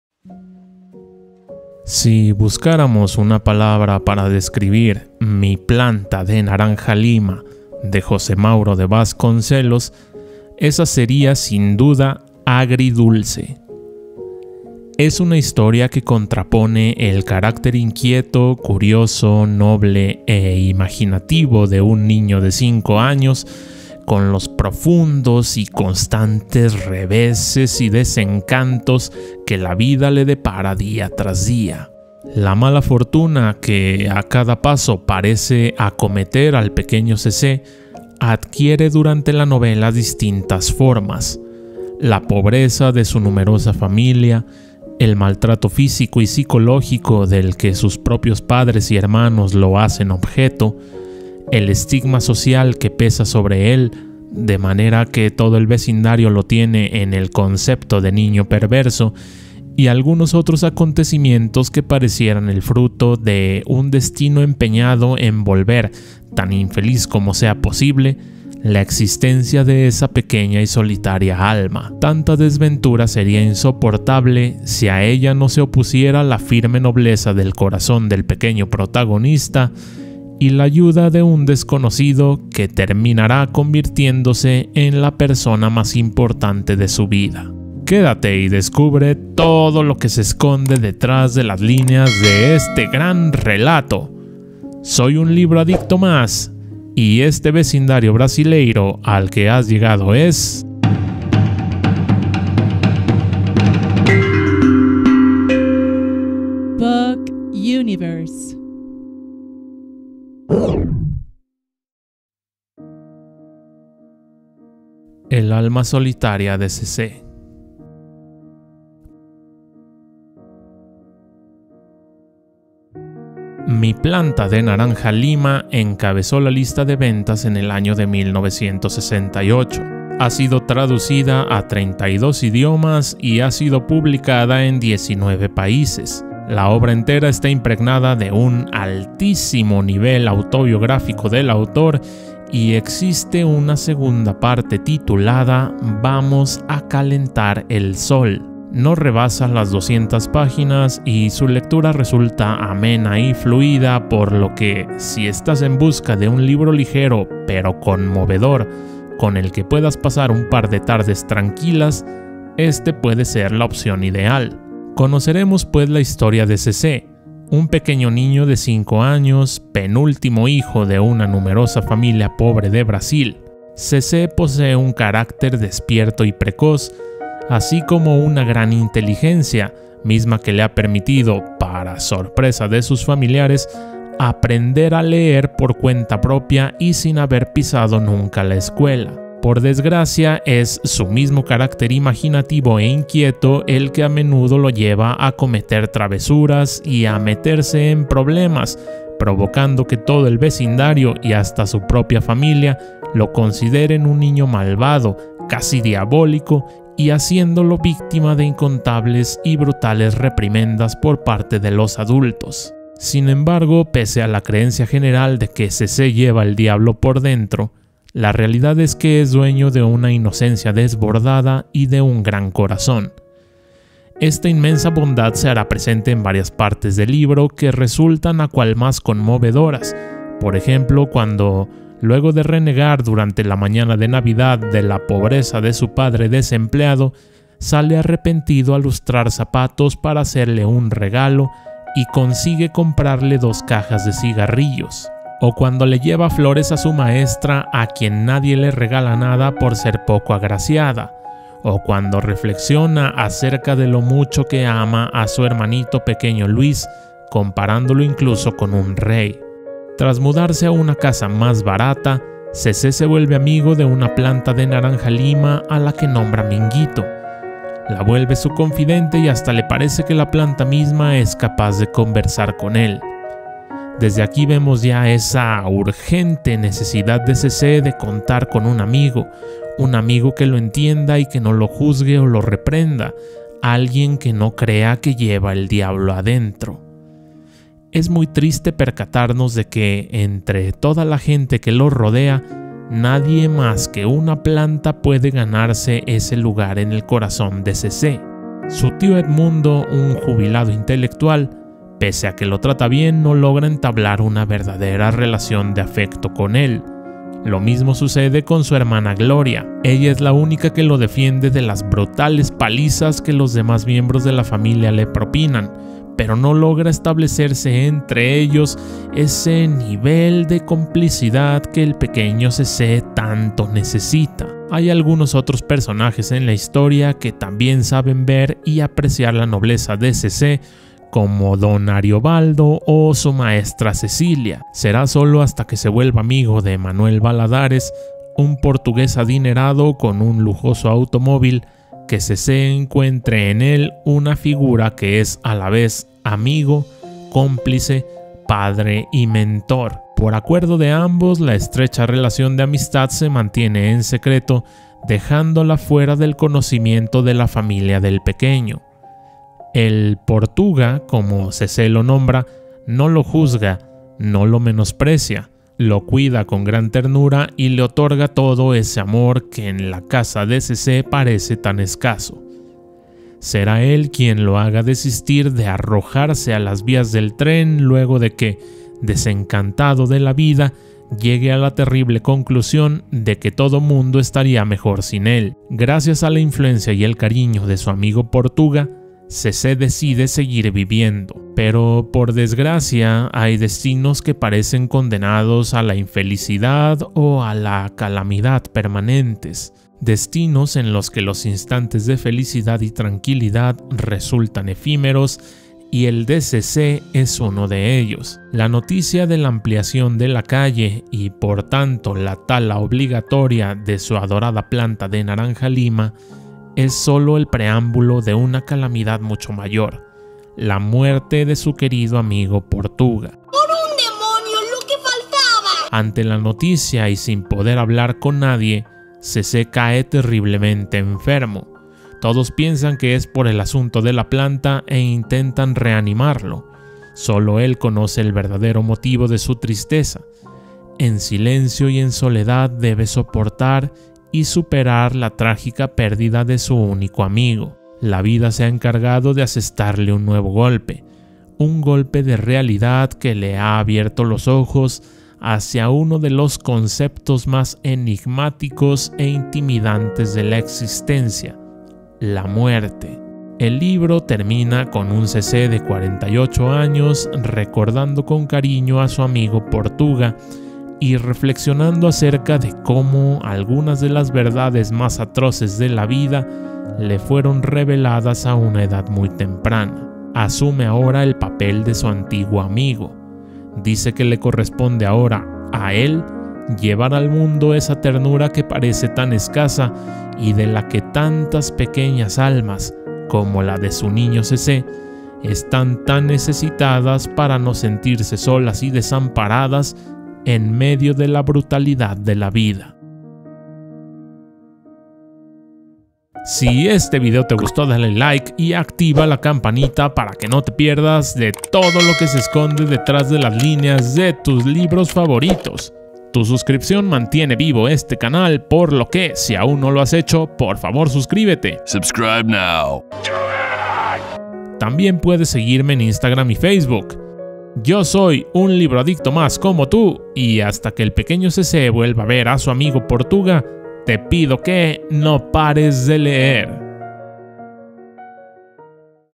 MI PLANTA DE NARANJA LIMA de JOSÉ MAURO DE VASCONCELOS
Mi planta de naranja-lima de José Mauro de Vasconcelos encabezó la lista de superventas en el año de 1968. Ha sido traducida a 32 idiomas y ha sido publicada en 19 países.
Si la catalogáramos en una sola palabra esa sería: AGRIDULCE.
Es una historia conmovedora en la que seguiremos los pasos del pequeño Zezé, un niño de cinco años, penúltimo hijo de una familia pobre de Brasil que muestra una inteligencia fuera de lo común así como un carácter despierto y precoz.
Por desgracia serán esas mismas cualidades las que lo lleven a realizar diversas travesuras y a meterse constantemente en problemas con los demás habitantes del vecindario quienes lo catalogan dentro del concepto de "niño perverso".
Sin embargo, Zezé posee un alma noble y un gran corazón, características que le permitirán hacer frente a las difíciles circunstancias que la vida va poniendo en su camino.
¡Descubre todo lo que se esconde detrás de este gran relato!